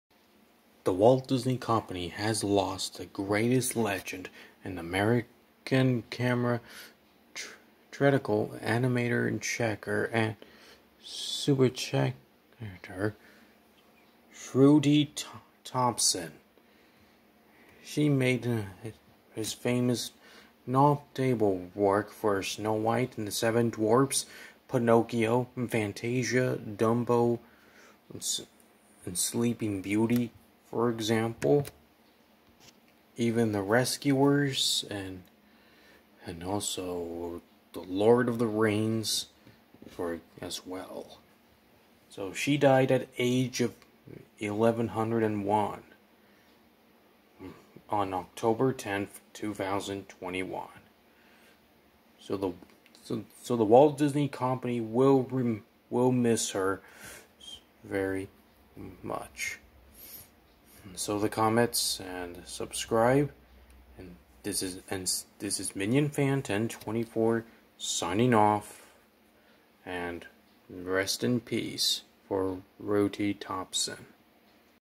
Ruthie Tompson (1910-2021)
The Disney legend American camera technician, animation checker and supercentenarian has died at age 111.